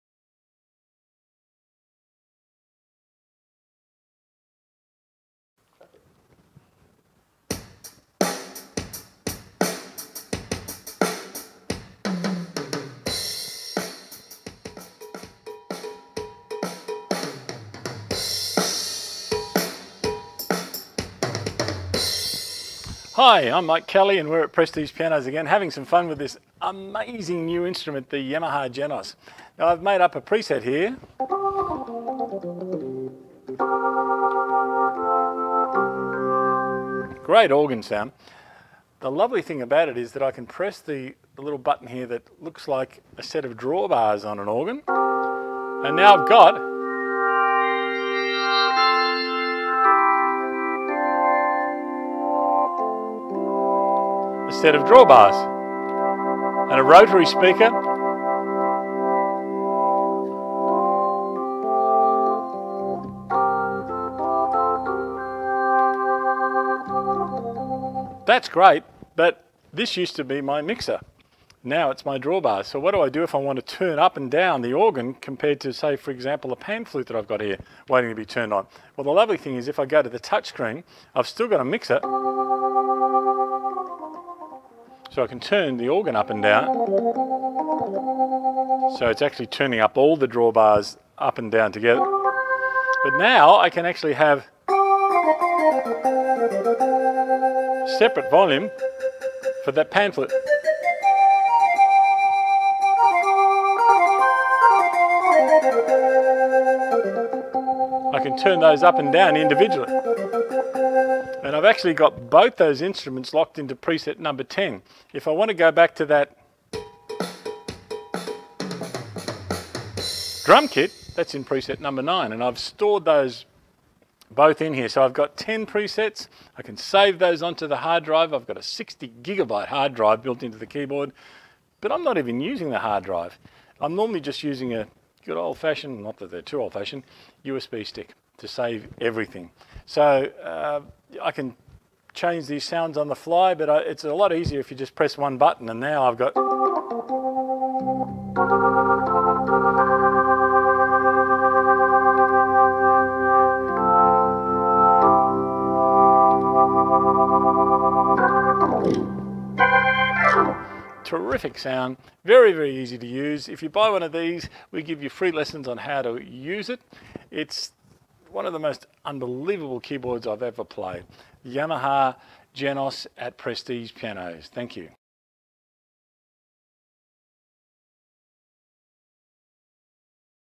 Yamaha Genos Digital Workstations keyboard
Demonstrating Manual Drum playing and Organ Drawbars with mixer